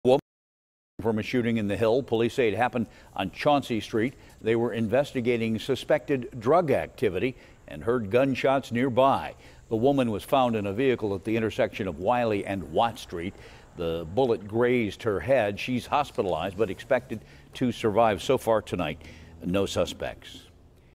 Police Investigate Multiple Shots Fired, Woman Injured In Hill District
Police are investigating after multiple shots were fired in the Hill District and a woman was injured.